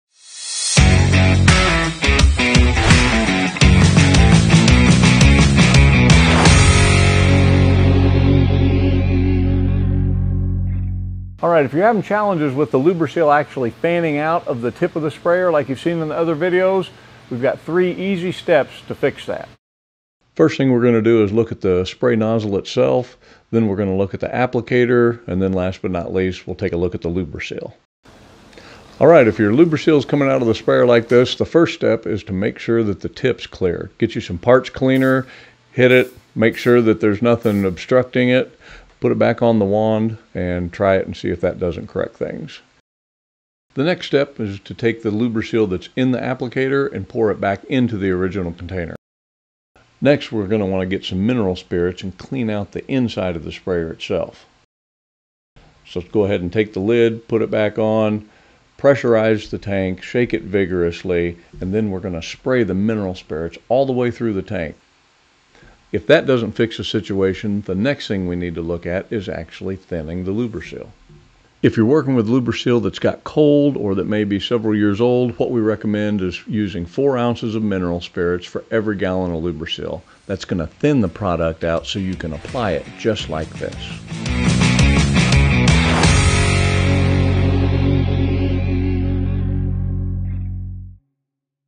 LUBRA-SEAL Application Troubleshooting
Welcome to our RHOMAR Troubleshooting Series! In this video, we focus on resolving common spray issues with LUBRA-SEAL when used with our Public Works Applicator (PWA). Whether you’re facing uneven spray patterns or a clogged nozzle, we’ve got you covered with easy-to-follow solutions.

What You’ll Learn:

Clearing the T-Jet Nozzle: Learn how to use brake cleaner to effectively clear blockages in the nozzle for a smooth, wide spray.
Cleaning the Applicator and Hose: Step-by-step instructions on using mineral spirits to clean the interior parts of the applicator and hose to ensure optimal performance.
Thinning Thickened LUBRA-SEAL: We’ll show you how to use mineral spirits to thin out LUBRA-SEAL, which has thickened over time, restoring its proper flow and functionality.
If you encounter further issues or have any questions, don’t hesitate to reach out.